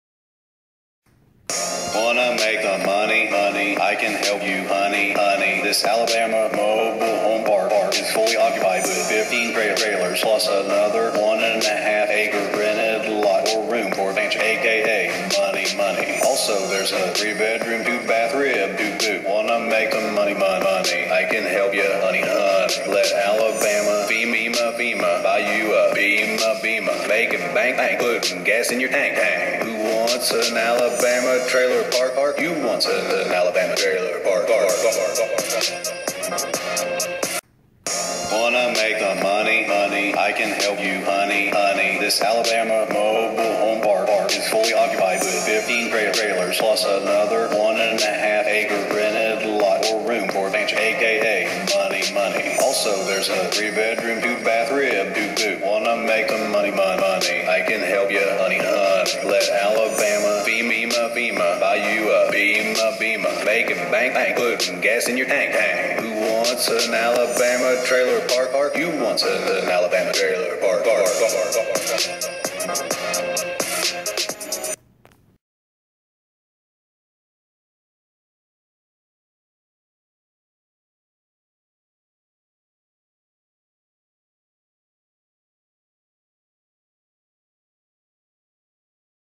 Mobile Home Park Centre Alabama Real Estate Investment Property
Attention investors! If you are looking for an income producing business, come see this beautiful mobile home park sitting on 4+/- acres with 15 rented trailers plus 1.5 acre rented lot and a 3 bedroom 2 bath house.